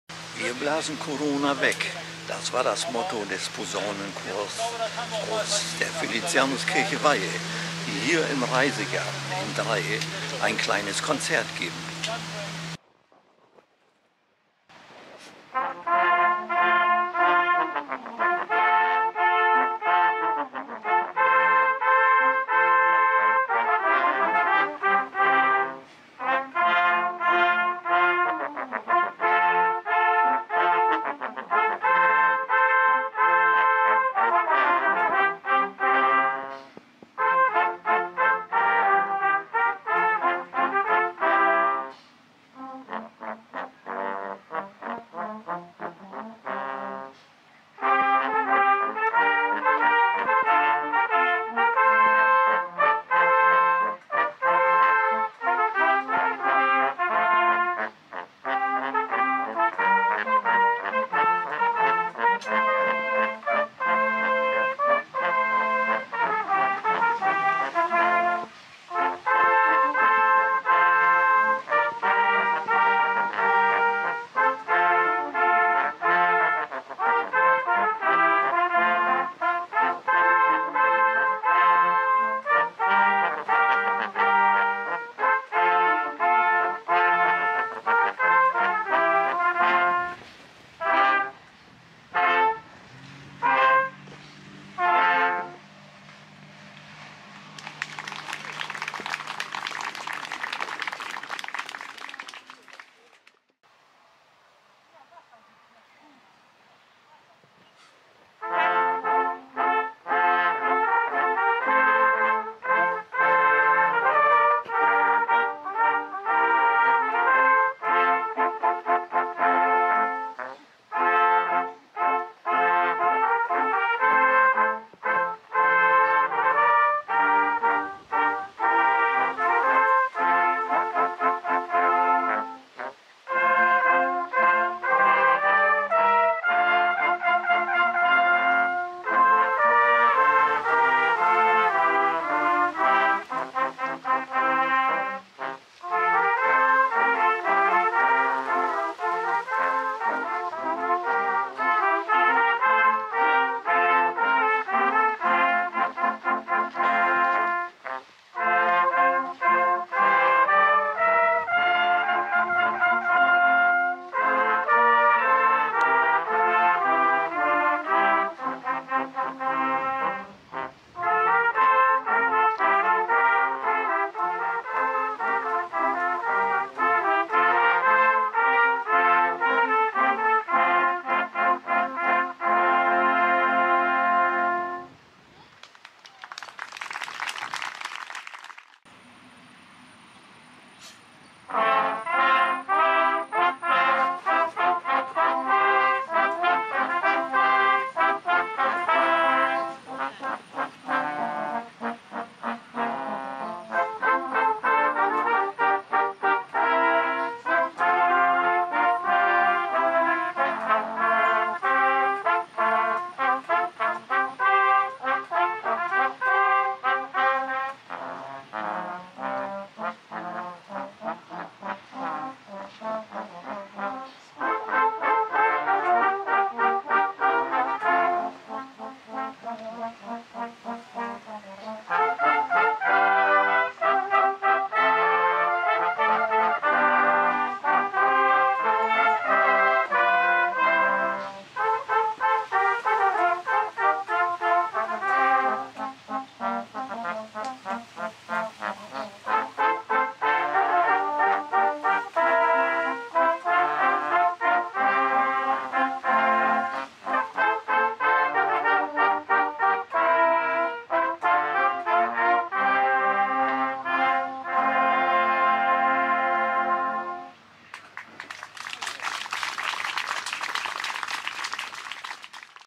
17/07/2021 - Wir blasen Corona weg (Konzert im Reisegarten Dreye )
Wir "blasen Corona weg" - Konzert vom Reisegarten Dreyhe